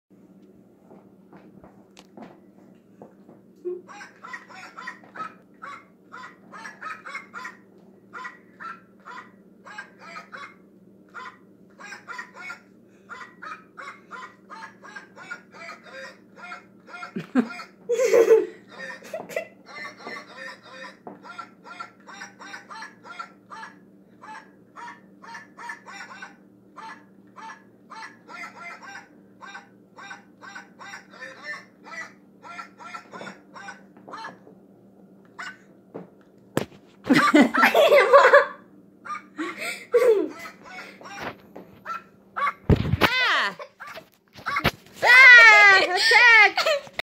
Presley and her singing dog puppet
Singing dog puppet?!